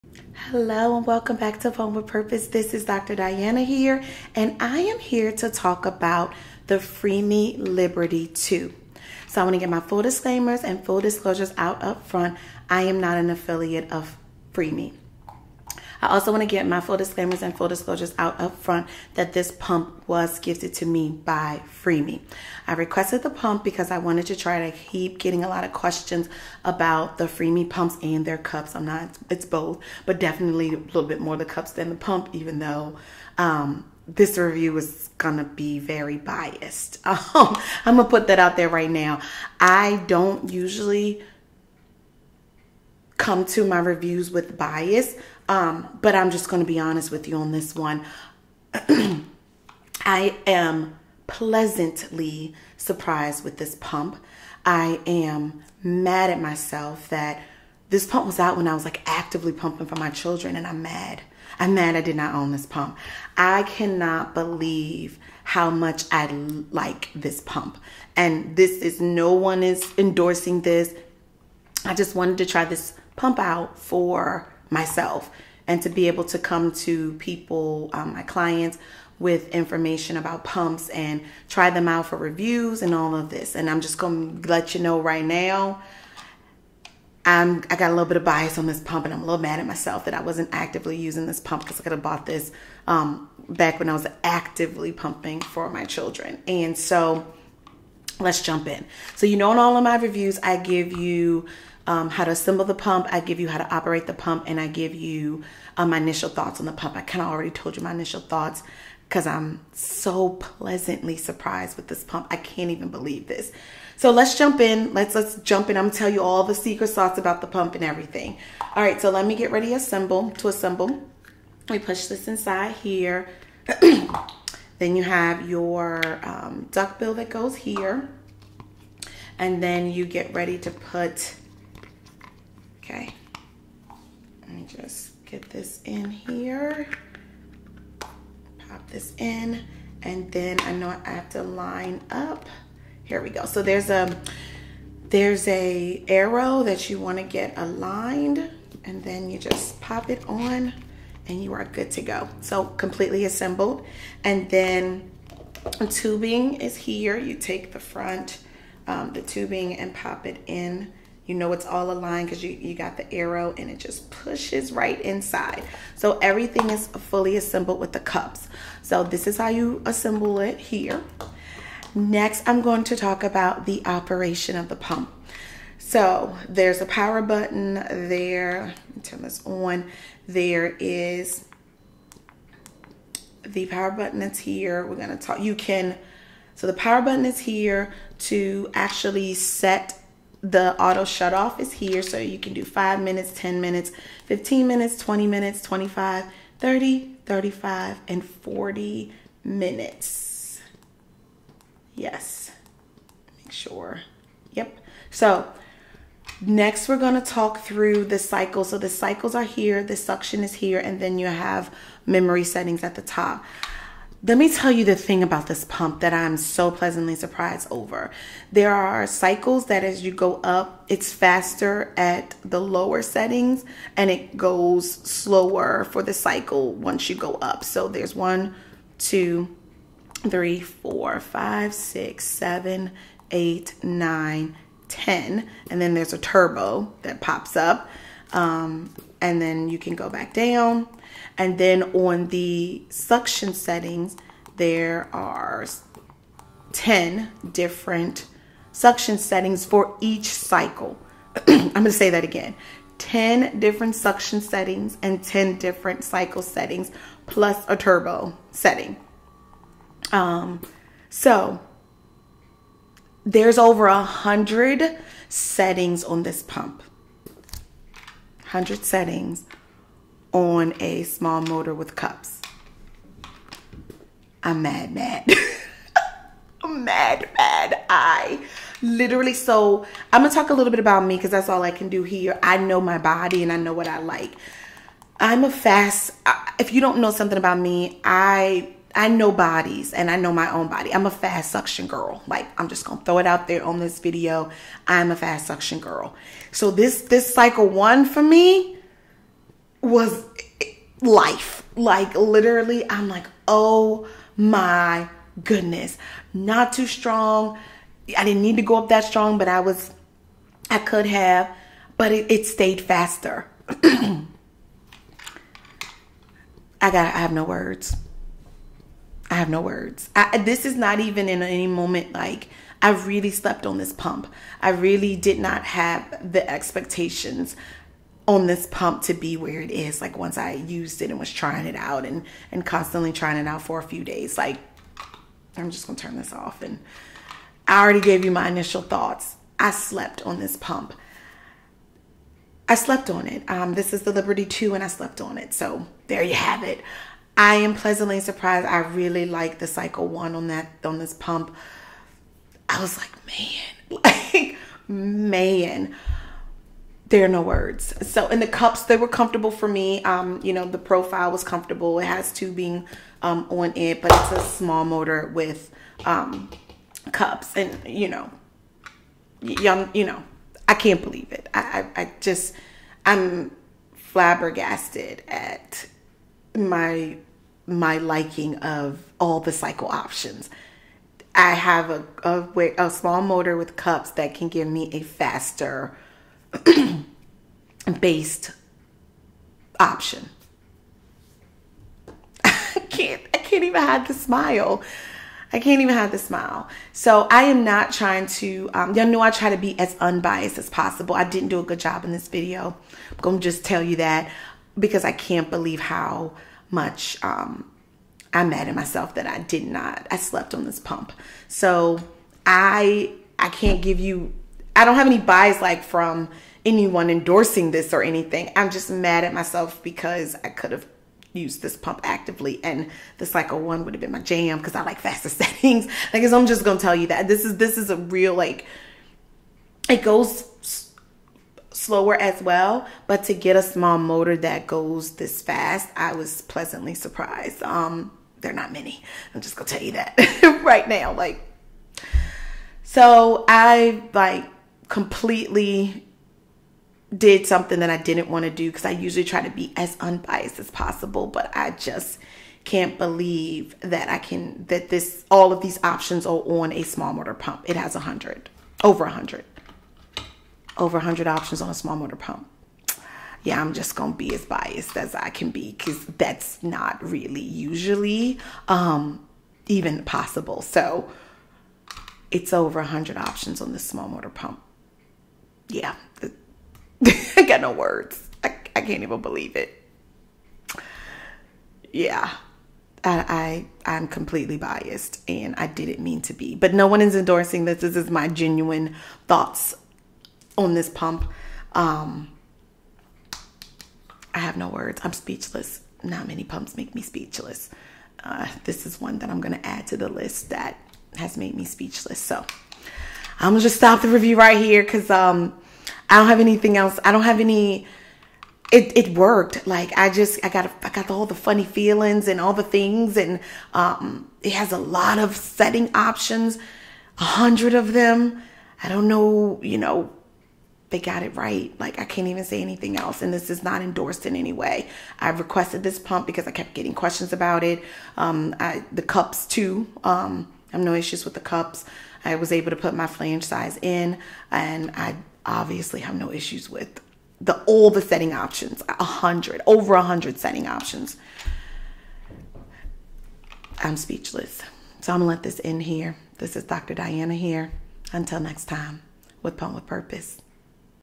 Freemie Liberty II Breast Pump Review
✅how to assemble breast pump
✅how to operate breast pump
✅initial thoughts of this breast pump